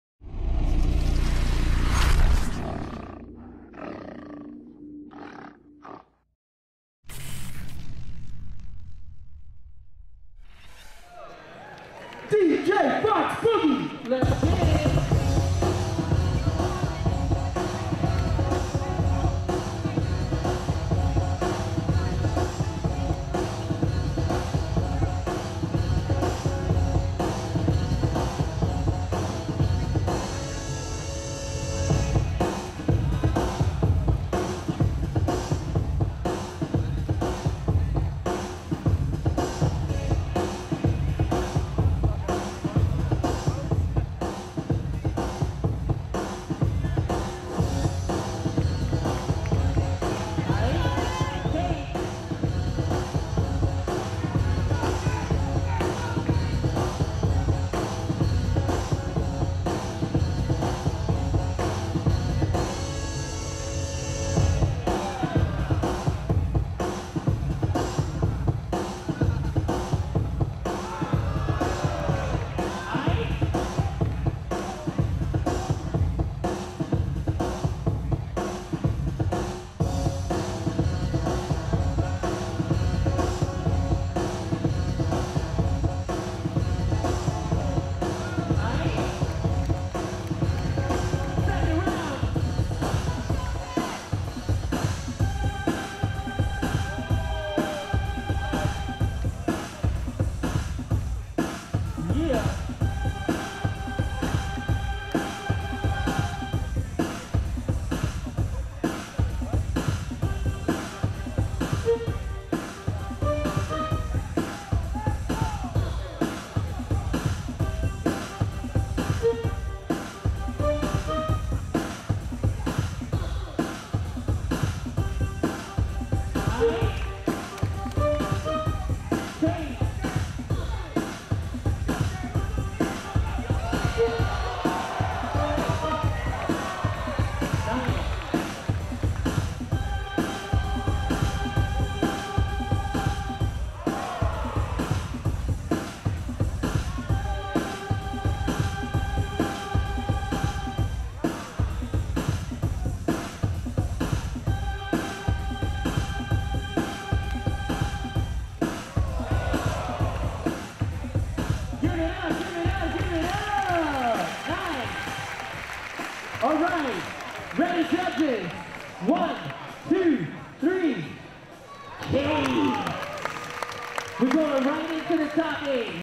Kate Vs Shortie Short | Top 16 B-Girl | Silverback Open 2016 | Pro Breaking Tour | BNC
Silverback Open Championship 2016

1 V 1 B-Girl Top 16

Winner
Kate

Judges
Swift Rock - Battle Squad (Germany)
Twixx - Massive Monkees (United States)
Dyzee - Supernaturalz (Canada)
Physix - Rivers (Korea)
Moy - Havikoro, Monster Bboys (United States)
Ronnie Ruen - Style Elements, Killafornia (United States)
Renegade - Soul Mavericks (U.K.)

DJs
Foxx Boogie

Emcee
Ivan The Urban Action Figure
Nemesis